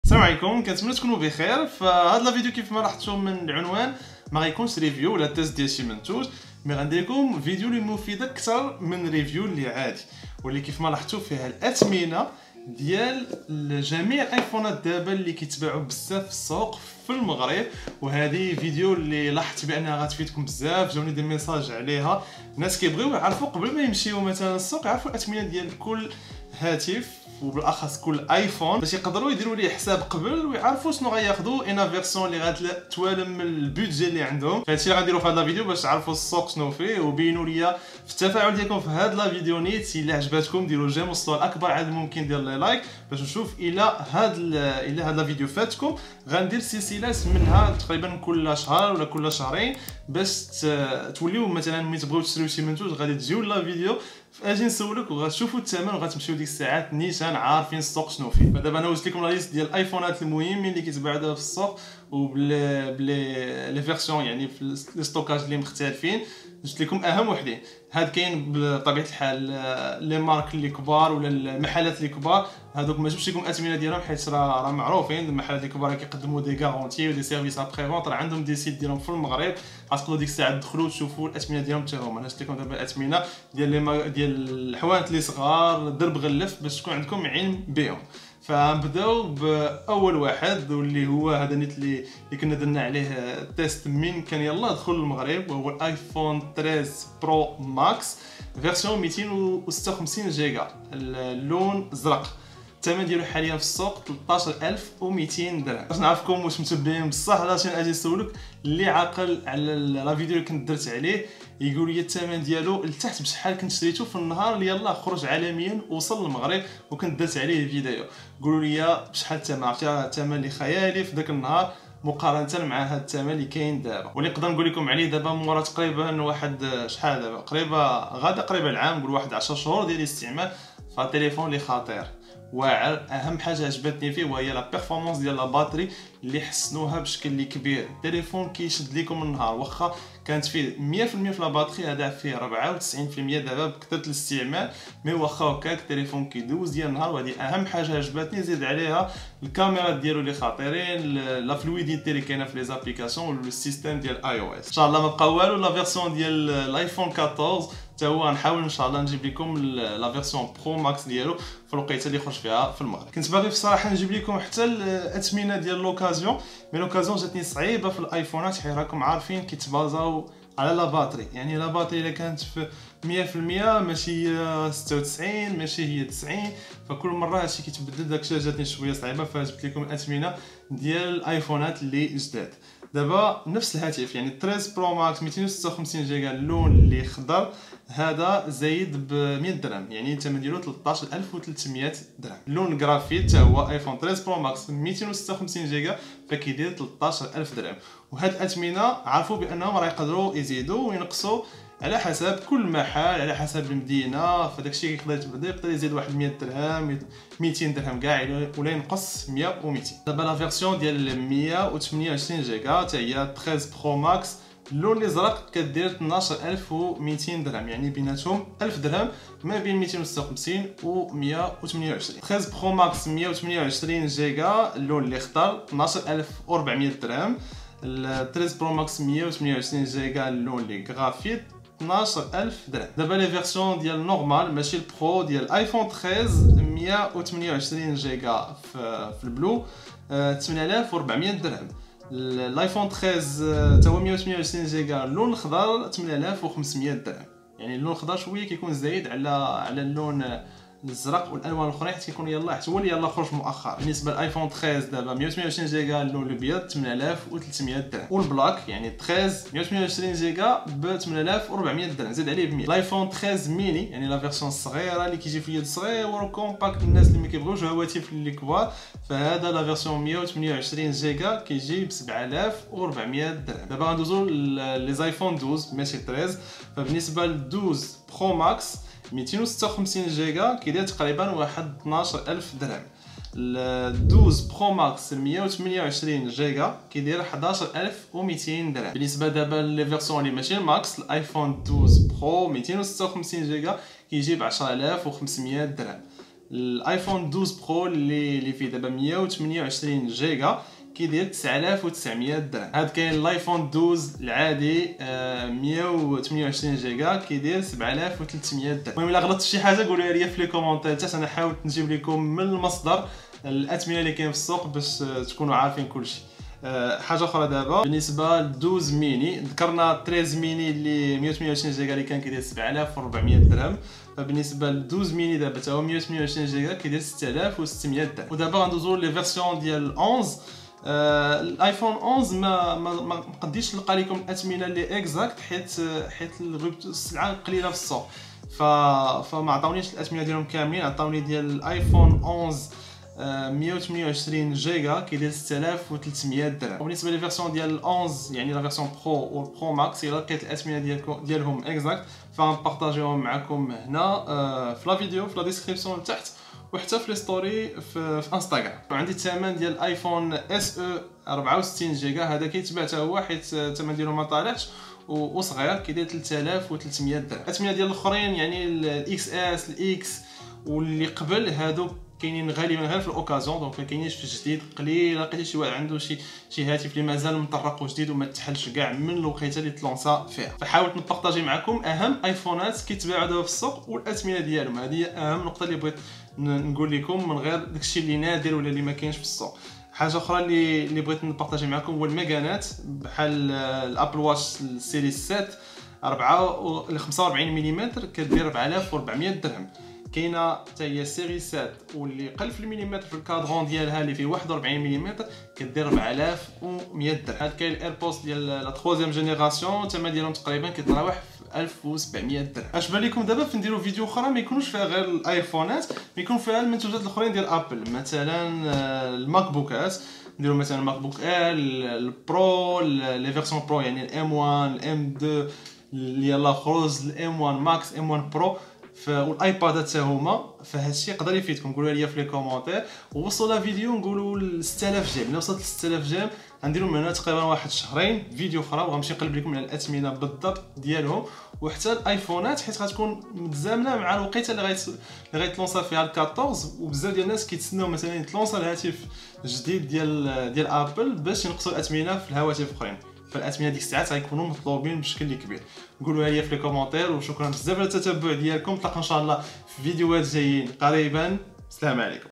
اثمنة جميع الأيفونات في المغرب 🇲🇦
Le Prix des iPhones au Maroc - Mois de Juillet 2022
أثمنة جميع الأيفونات الموجودة بكثرة في المغرب لشهر يوليوز 2022